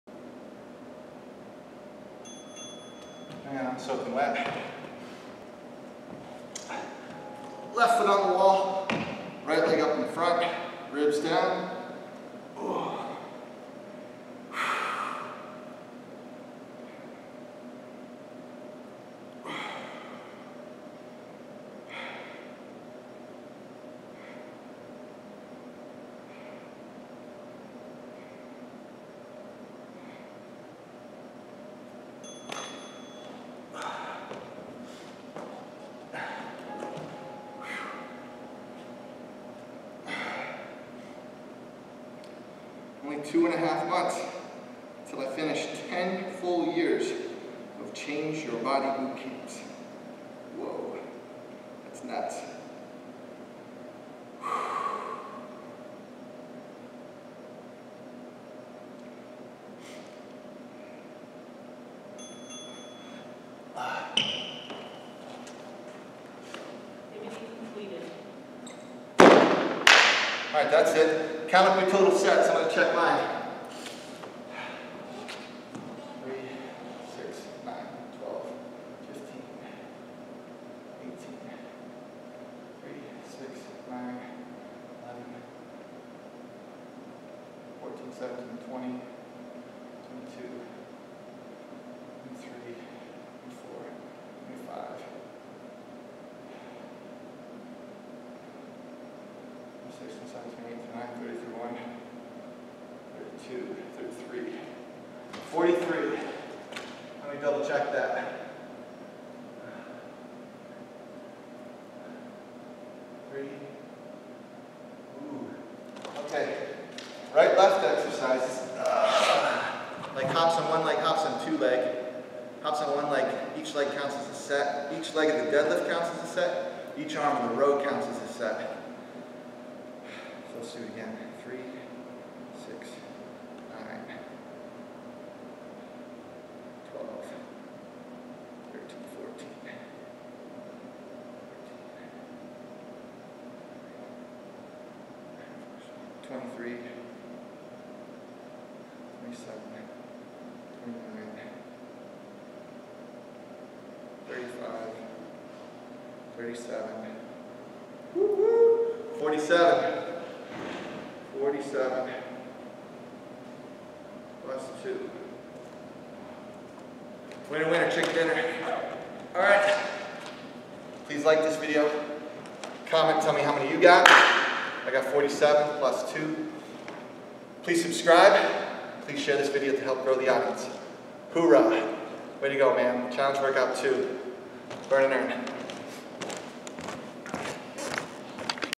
Newton Boot Camp: 2018, P11, Full Length, Challenge Workout #2 (W-A), Day 2, p2
to learn more.

Workout A (modified), Day 2:  complete Max Sets in ~28.5min!

Strength: complete Max Sets in ~28.5min; G=45 sets; 25-45%BW.

4min (3:1), Max Sets, G=6
A1.  Alt. Shoulder Tap x10e
A2.  Linear Hurdle Hops – R+L x5e

5min (3:2), Max Sets, G=6
B1.  Towel Body Saw x12
B2.  Squat Jacks x20

6.25min ((3.75:2.5), 15:10, 3rd circuit, 9sets, Max Reps
C1.  DB Squat Jump w/ Reset
C2.  1-DB Tall Kneel Arnold Shoulder Press - R
C3.  1-DB Tall Kneel Arnold Shoulder Press - L

5min (4.5:0.5), Max Sets, G=6
D1.  DB Row – R+L x8
D2.  Push Up to High Plank March (4sets + PR) x10

Conditioning:  3.25min (2:1.25), Max Sets, G=10
Line Hops (linear x10e, lateral x10e, square jumps 2x5e, fig8 2x5e, 2-leg linear x10e)

5min (4.5:0.5), Max Sets, G=8
E1.  2-DB SLDL – R+L x8e
E2.  TRX Half Kneel T-Raise x8

*Coach Mike did 47 total sets; G=45 total sets

Push Up Calf, R / L
½ Kneel Quad + Hip Flexor Push @ Wall, R/L